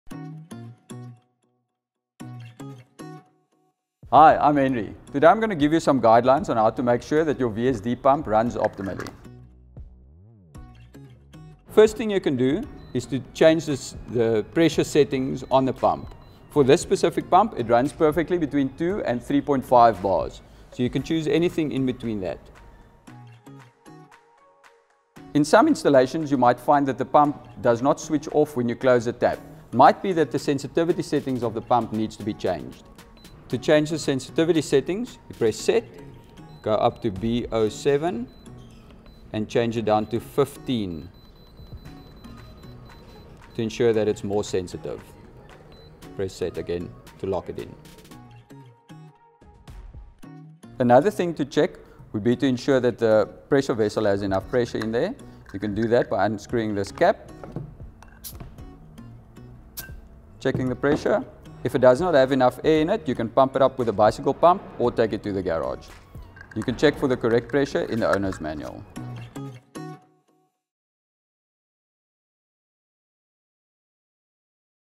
DIY with JoJo_How to ensure your 0.75kW VSD Pump Runs Optimally
Ensure optimal performance for your 0.75kW VSD Pump with our easy guide! Learn how to check the pump's pressure and shut-off sensitivity to keep your water system operating at its best.